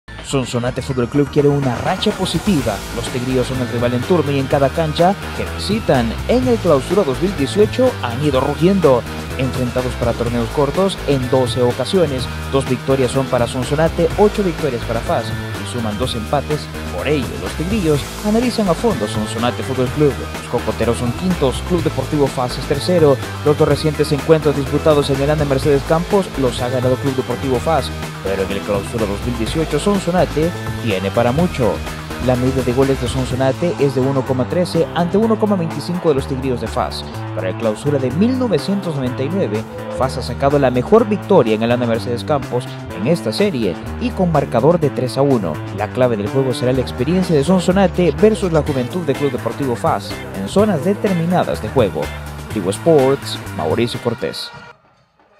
Previa Sonsonate vs FAS Jornada 9 | Tigo Sports
¡Hoy toca noche de fútbol!
Así se enfrentan hoy a las 7:00 PM Sonsonate Fútbol Club y Club Deportivo FAS EN VIVO y en HD por Tigo Sports.